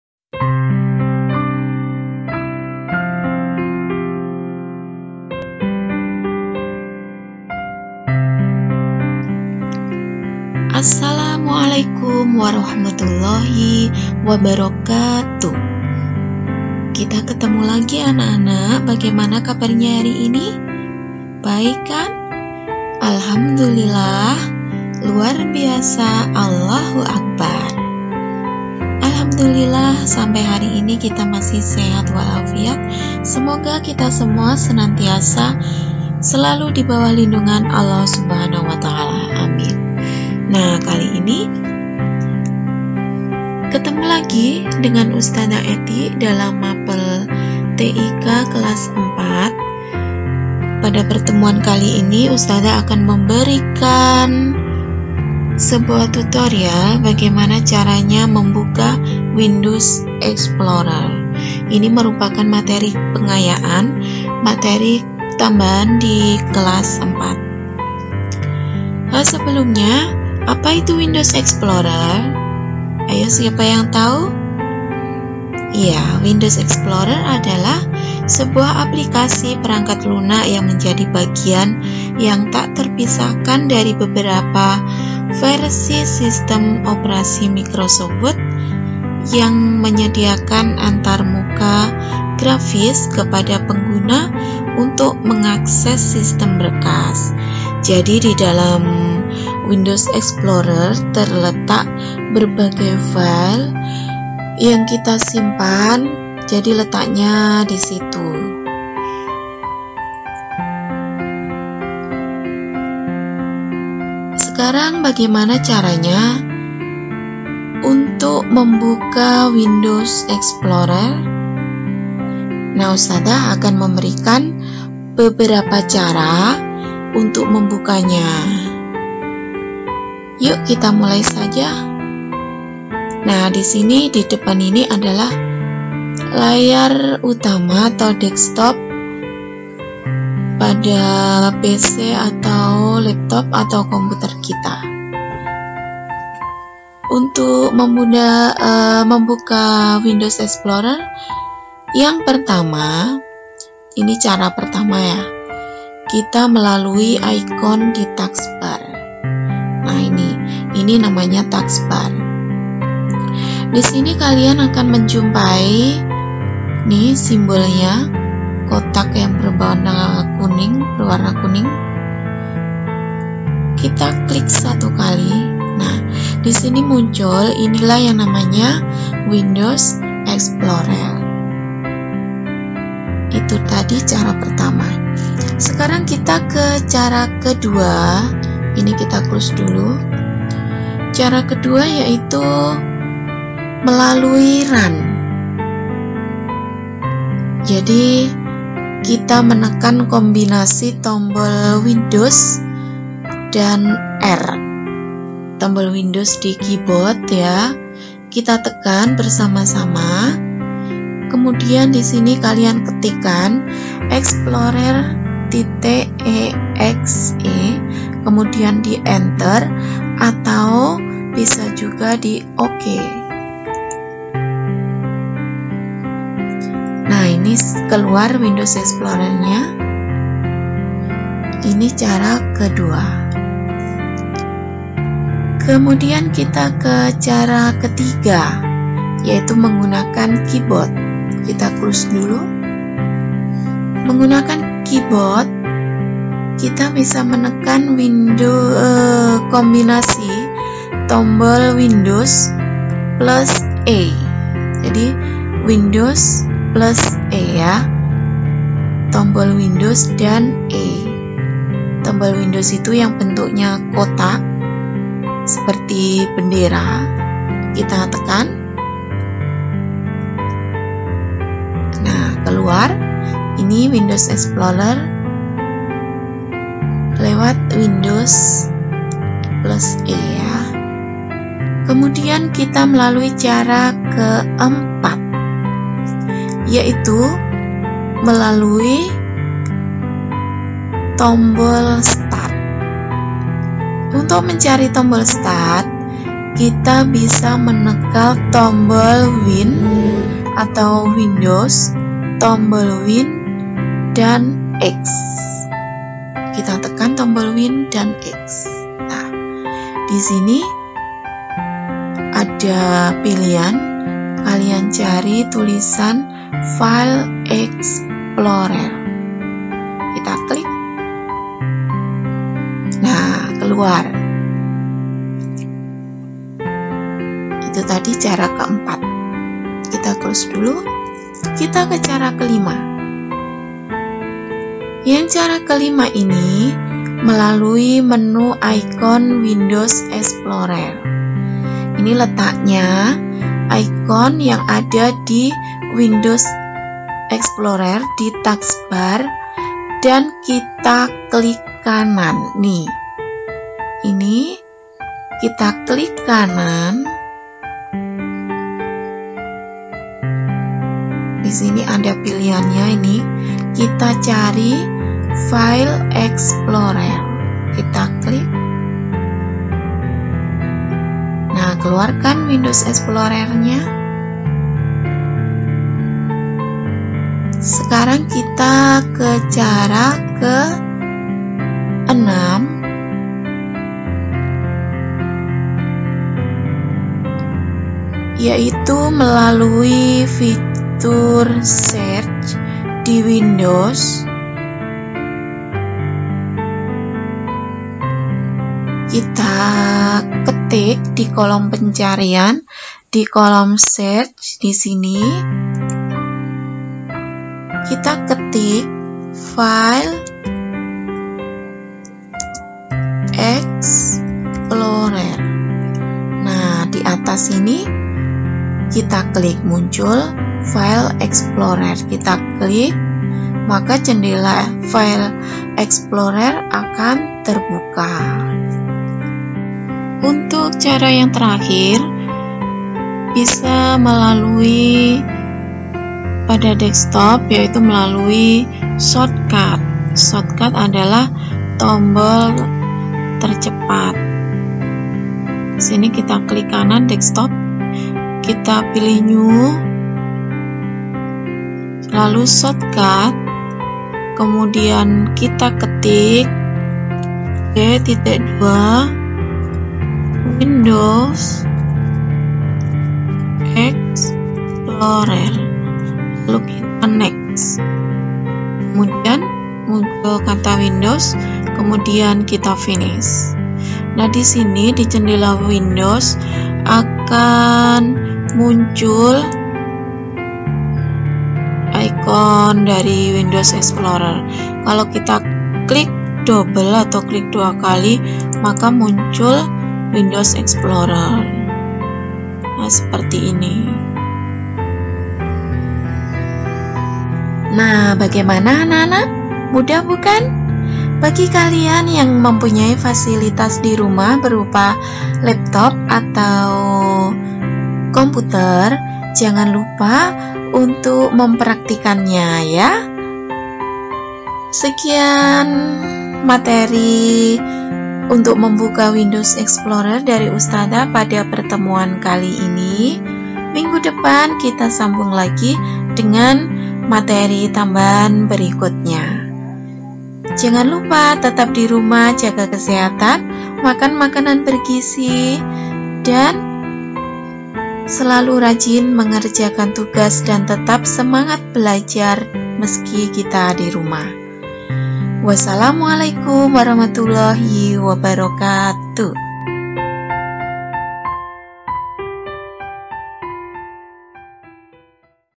PEMBELAJARAN DARING TIK KELAS 4-CARA MEMBUKA WINDOWS EXPLORER-elifia
Pembelajaran explorer TIK kelas 5
Materi: cara membuka windows explorer